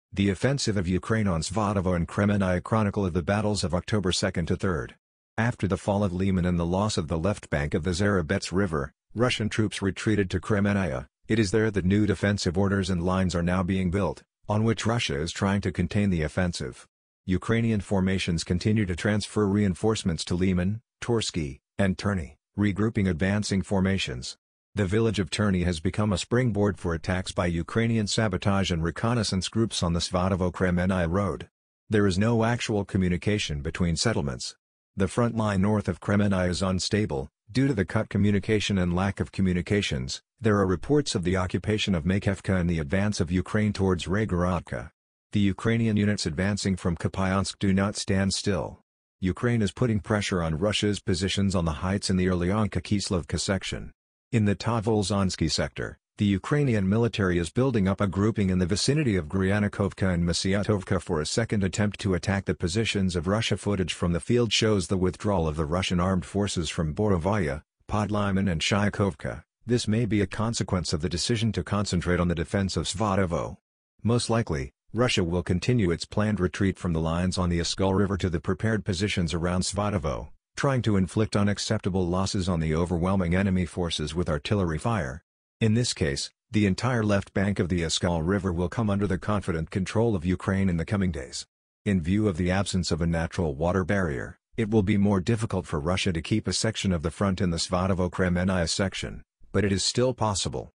War in Ukraine. The attack on Svatovo and Kremennaya Chronicle of the battles of October 2-3
War is tragedy on both sides.

The attack on Svatovo and Kremennaya Chronicle of the battles of October 2-3.

After the fall of Liman and the loss of the left bank of the Zherebets River, Russian troops retreated to Kremennaya: it is there that new defensive orders and lines are now being built, on which Russia is trying to contain the offensive.

Ukrainian formations continue to transfer reinforcements to Liman, Torskoye and Terny, regrouping advancing formations.

The village of Terny has become a springboard for attacks by Ukrainian sabotage and reconnaissance groups on the Svatovo-Kremennaya road. There is no actual communication between settlements.

The front line north of Kremennaya is unstable: due to the cut communication and lack of communications, there are reports of the occupation of Makeevka and the advance of Ukraine towards Raygorodok.

The Ukrainian units advancing from Kupyansk do not stand still. Ukraine puts pressure on the positions of Russia on the heights in the Orlyanka-Kislovka section.

In the Tavolzhansky sector, the Ukrainian military is building up a grouping in the vicinity of Gryanikovka and Masyutovka for a second attempt to attack the positions of Russia

Footage from the field shows the withdrawal of the Russian Armed Forces from Borovaya, Podliman and Shiykovka: this may be a consequence of the decision to concentrate on the defense of Svatovo.

Most likely, Russia will continue its systematic retreat from the lines on the Oskol River to prepared positions around Svatovo, trying to inflict unacceptable losses on the enemy's superior numbers with artillery fire.

In this case, the entire left bank of the Oskol River will come under the confident control of Ukraine in the coming days. In view of the absence of a natural water barrier, it will be more difficult for Russia to keep a section of the front in the Svatovo-Kremennaya section, but it is still possible.